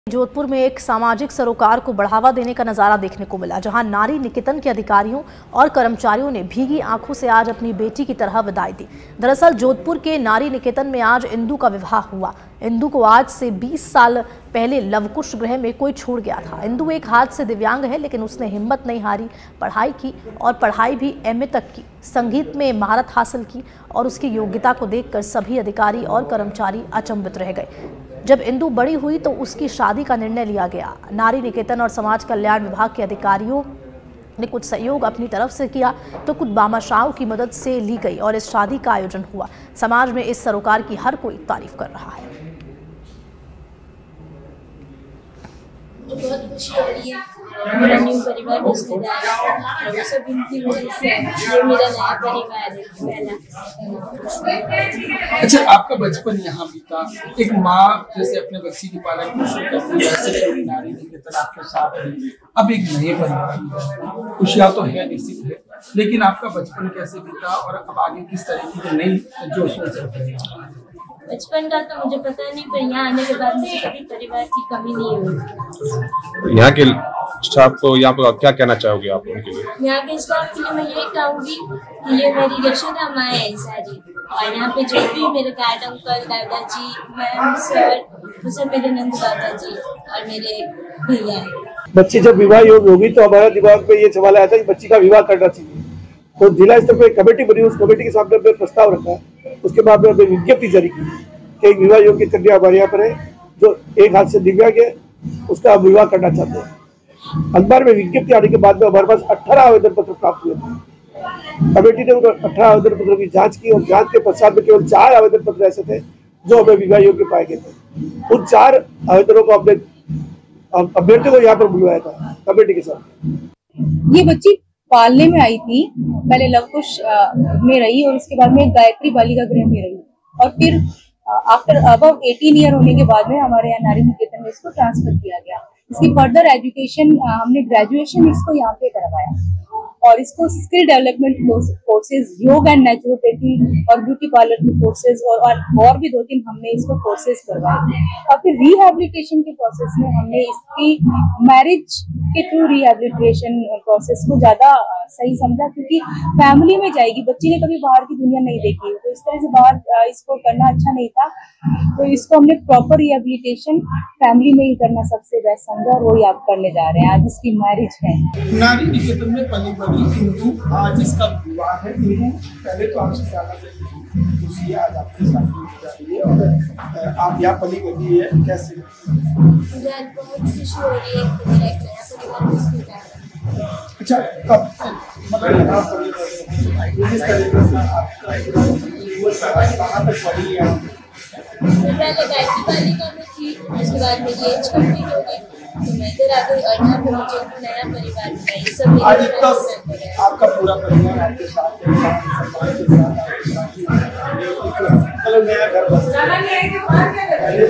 Jodhpur: नारी निकेतन ने निभाया सामाजिक सरोकार, अधिकारियों और कर्मचारियों ने करवाया इंदु का विवाह
About Video: Jodhpur: नारी निकेतन ने निभाया सामाजिक सरोकार, अधिकारियों और कर्मचारियों ने करवाया इंदु का विवाह